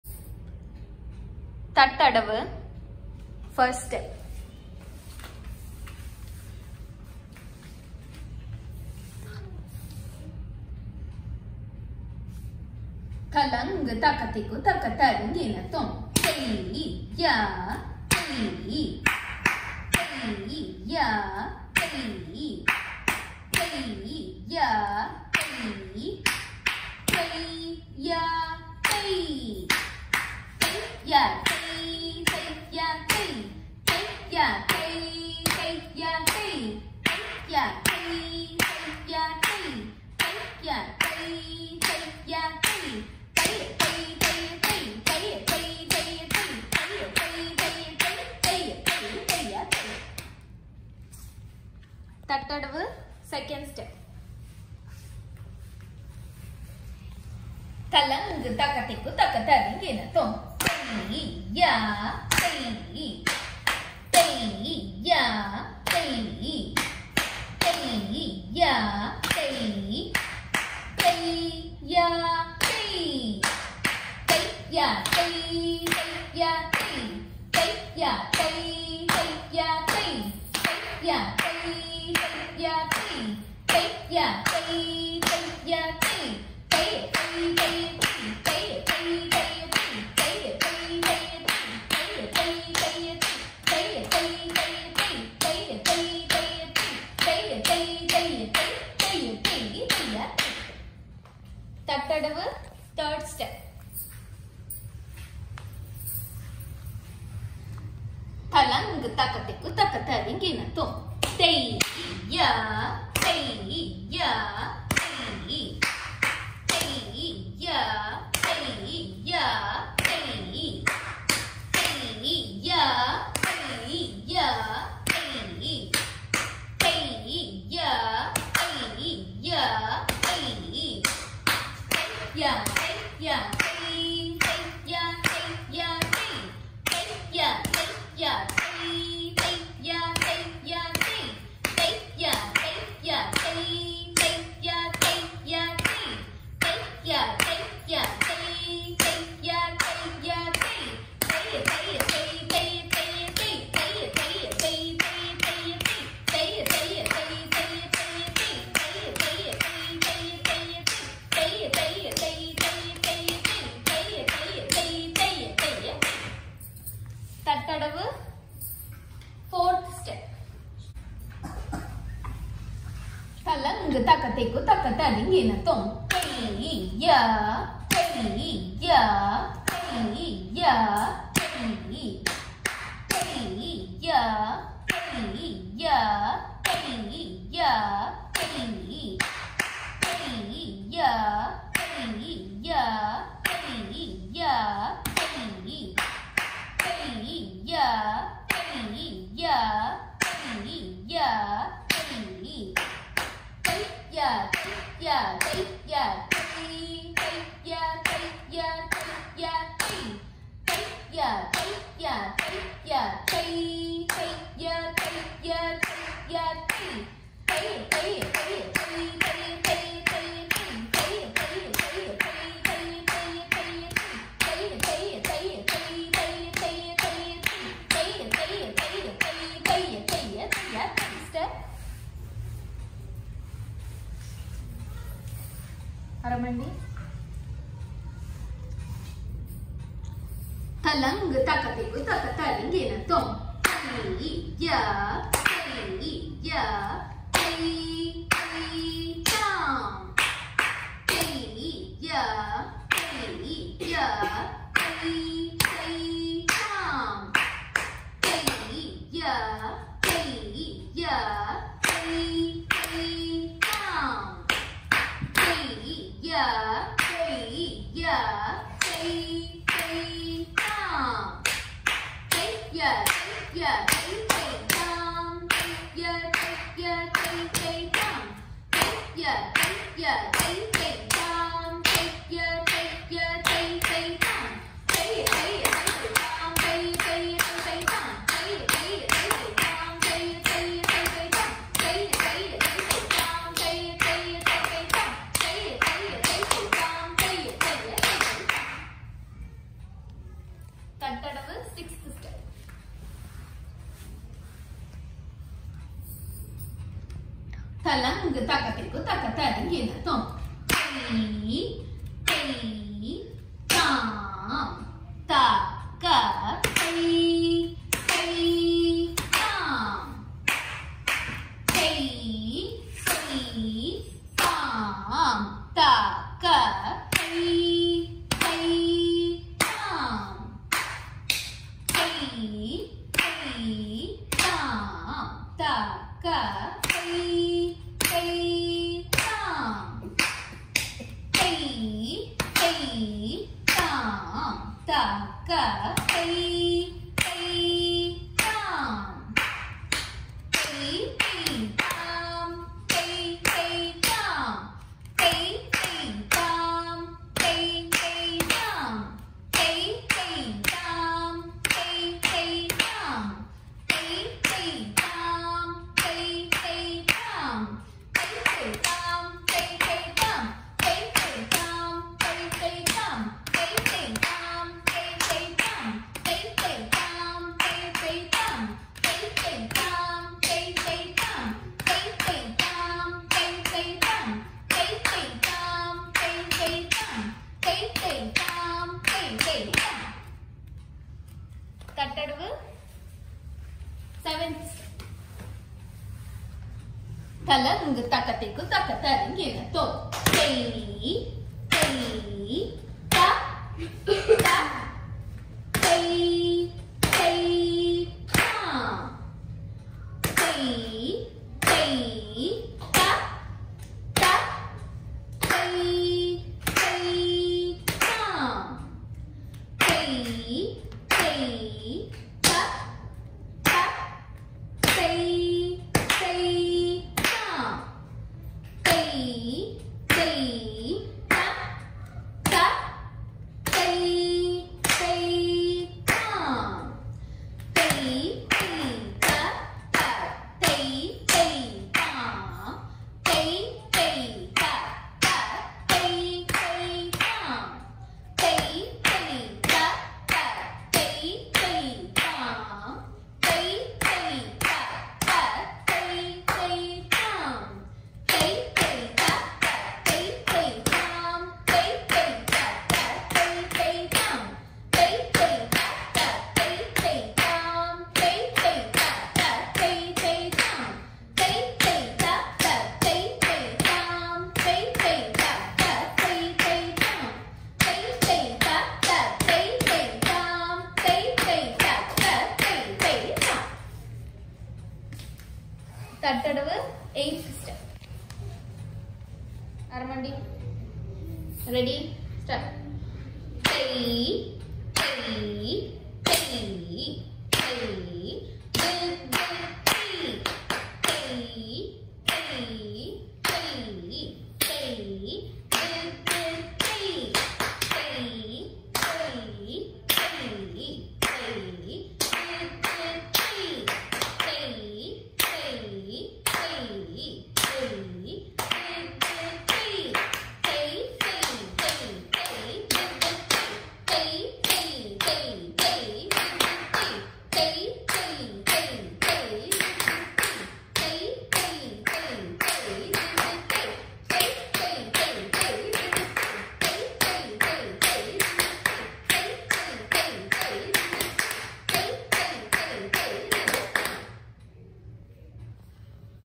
Thattadavu 1 to 8 Steps | Bharatanatyam
Reference video for Thattadavu 1 to 8 steps in all 3 speeds.
Beginners lesson.
Bharatanatyam lessons by Guru Shylu Winston